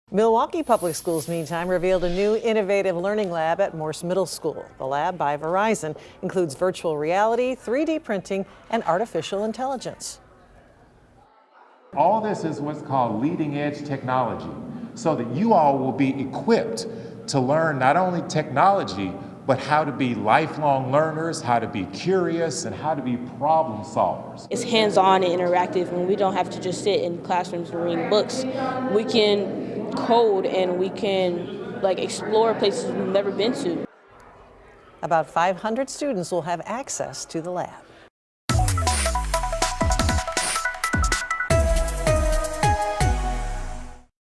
MPS reveals new learning lab at Morse Middle School
The lab, by Verizon, includes virtual reality, 3Dprinting and artificial intelligence.